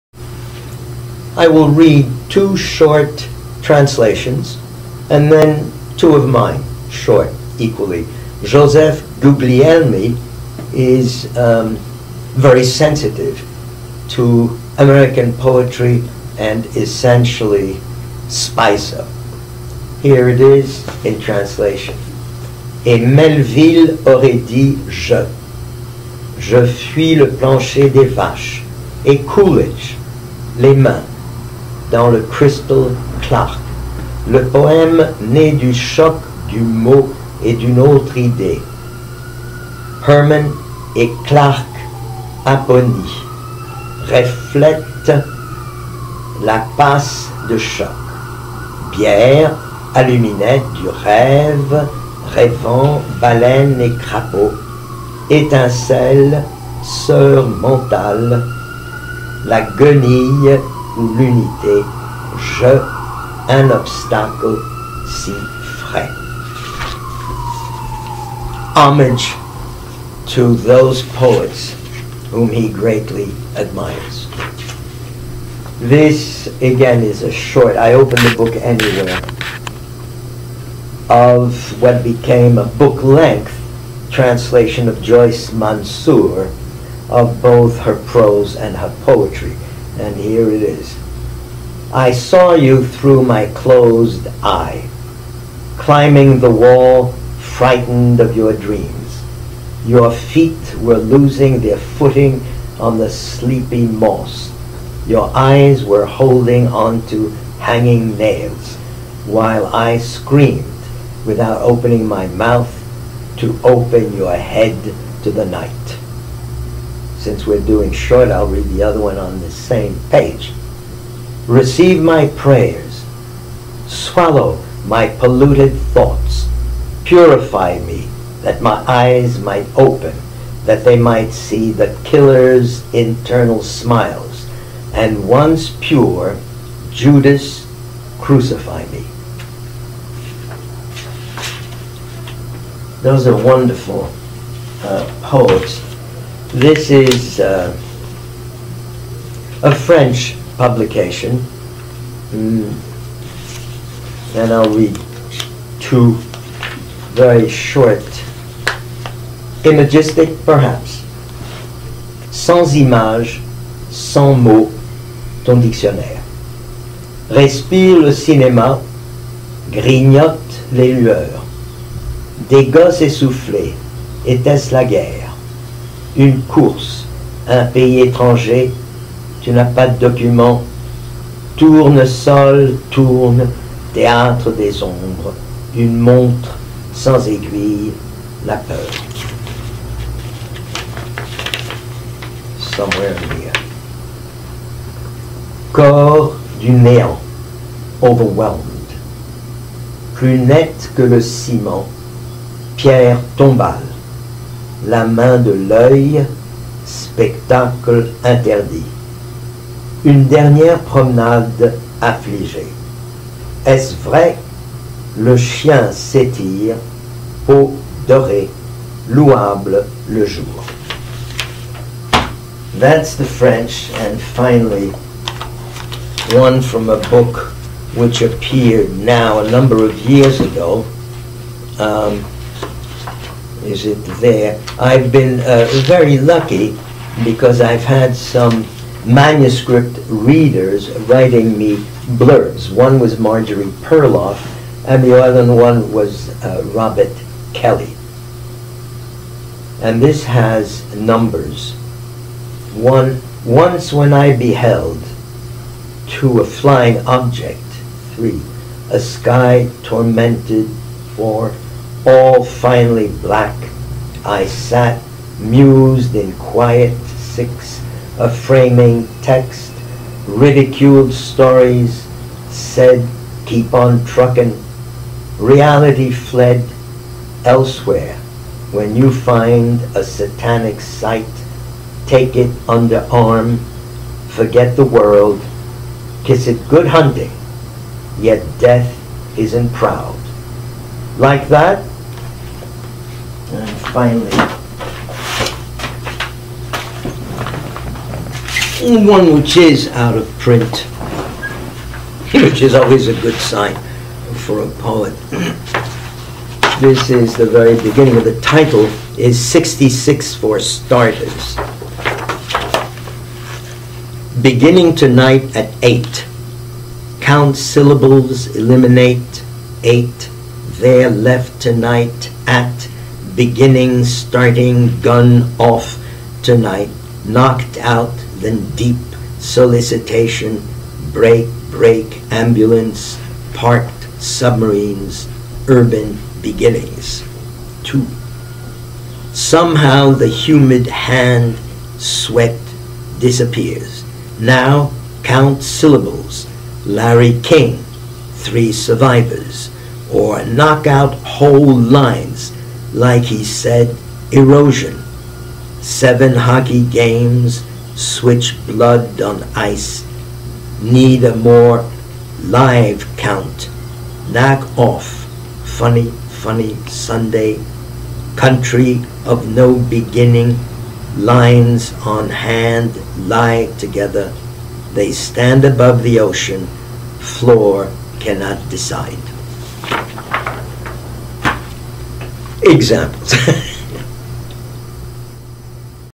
Readings by Serge Gavronsky
Serge Gavronsky, poet, novelist, translator and Professor of French at Barnard College, reads selections from Joseph Guglielmi, Joyce Mansour and from his own poetry.

00:22-01:14 Joseph Guglielmi
01:33-02:20 Joyce Mansour
02:41-03:41 Gavronsky (in French)
04:12-06:41 Gavronsky (in English)